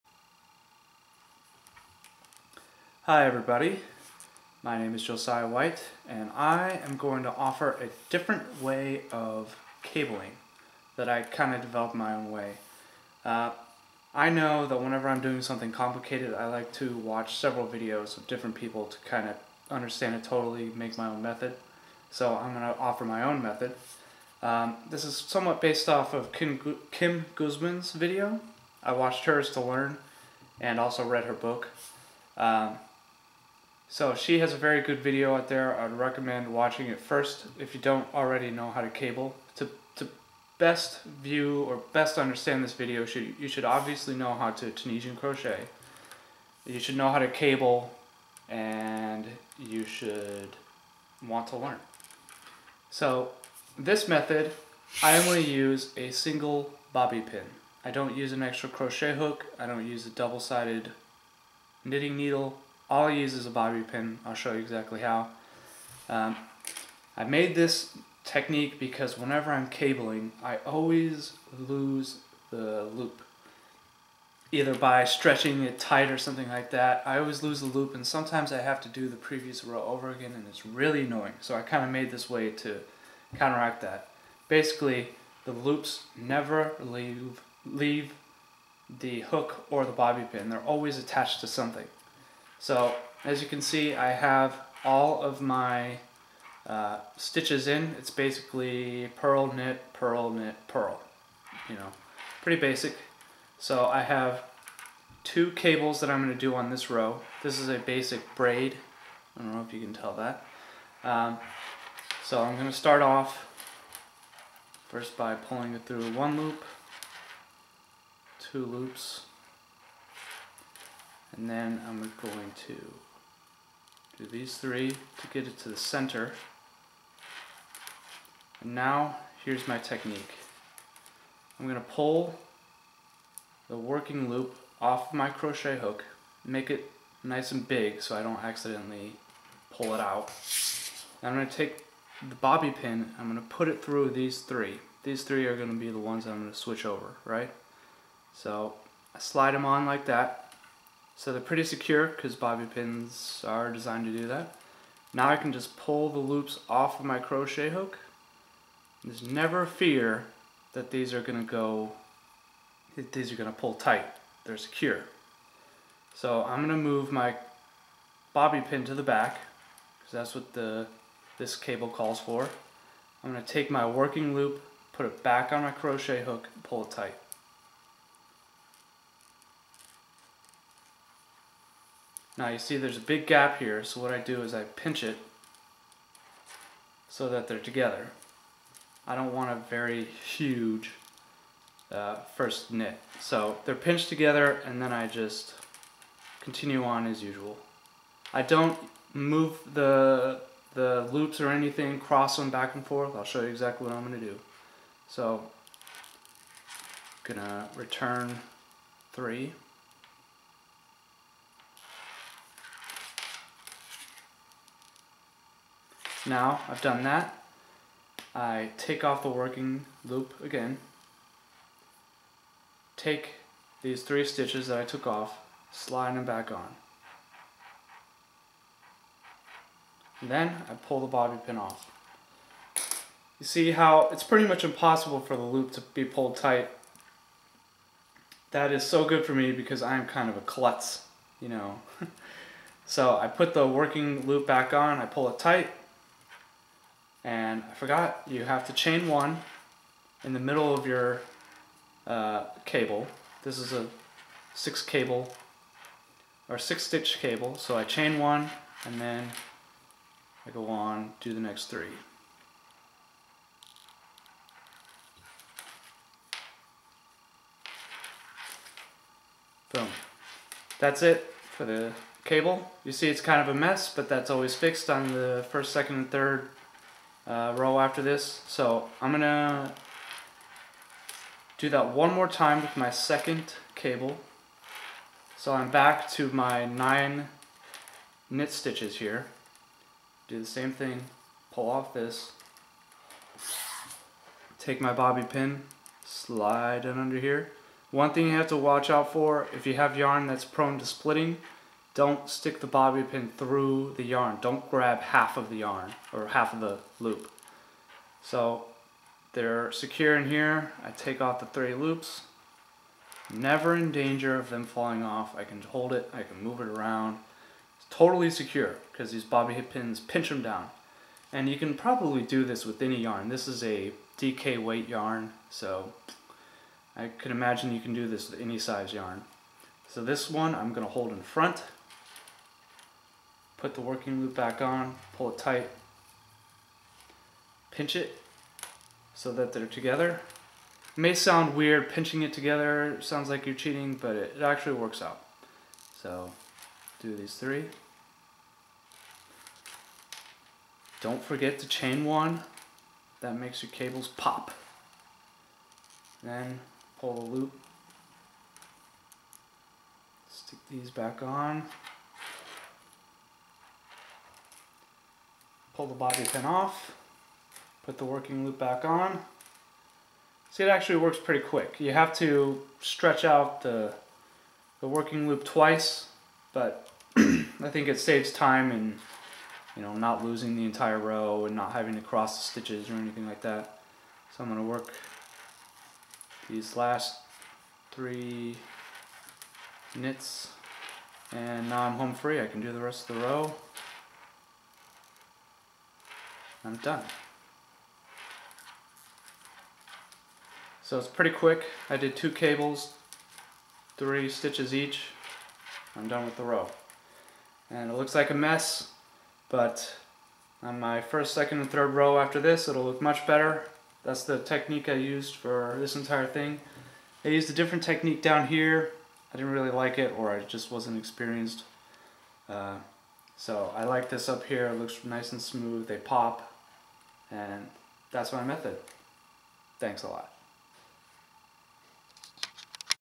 Tunisian crochet cabling using one bobby pin
This method of Tunisian cabling uses just one bobby pin and guarantees you don't drop a stitch or pull a loop off